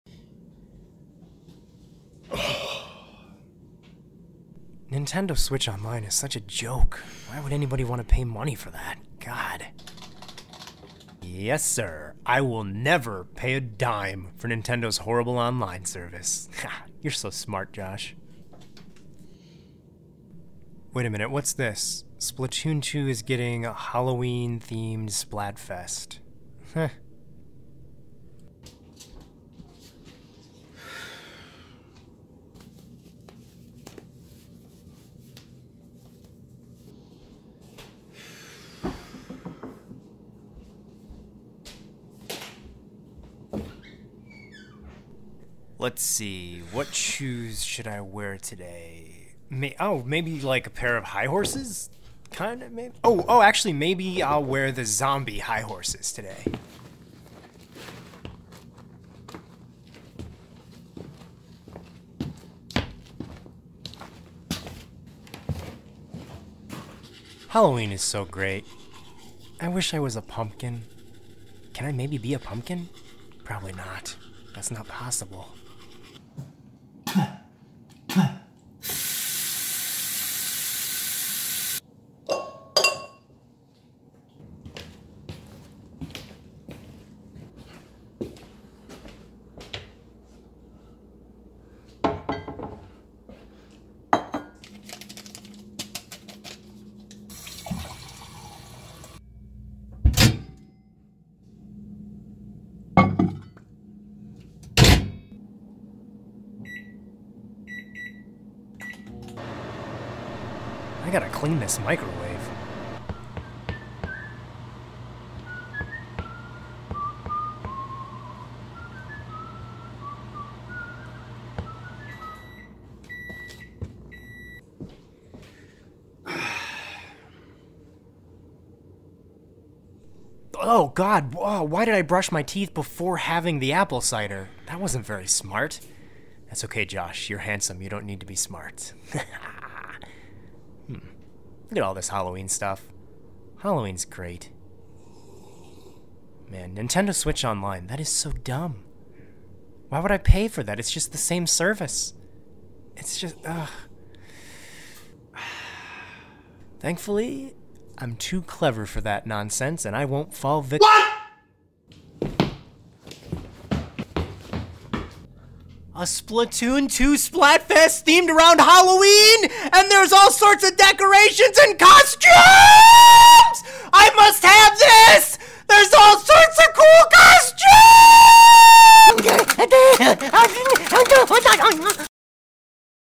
A Halloween Splatfest in Splatoon 2...WHAT?!
Halloween Splatfest is coming to Splatoon 2. Big deal I don't care.